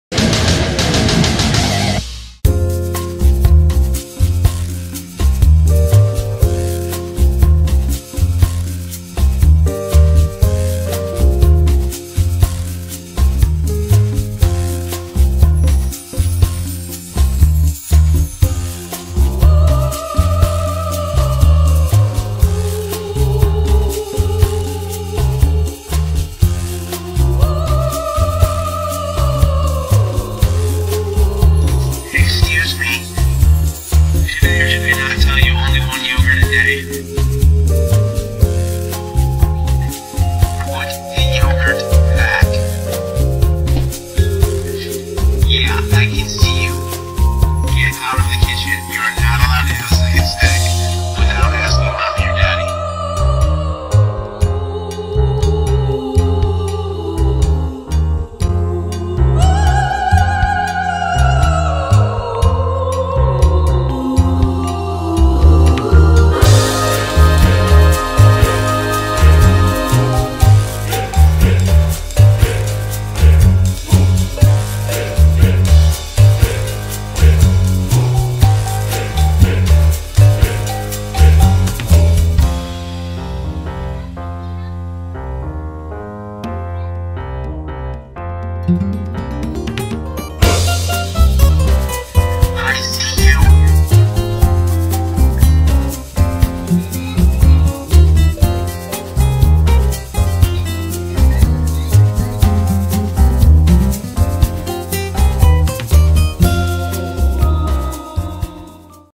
Raw Footage: Watching the Kids with the EZVIZ Mini 360 Plus Camera
I'm a big fan of knowing what's going on in my house when I'm not there, but I'm also a fan of straight-up spying on our mischievous daughters.